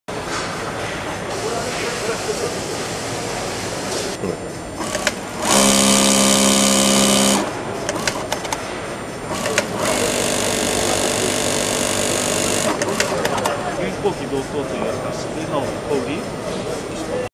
Juki DDL-8700-7 Industrial Sewing Machine
Juki DDL-8700-7 Single Needle Lockstitch with Automatic Under Bed Trimmer Demo video.